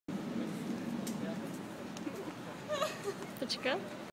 statnice.avi
Zamrznutí přirozených úsměvů aneb krutý boj za pěknou fotku, jejíž úsilí však zhatilo video za fotku převlečené.
Brno, 2012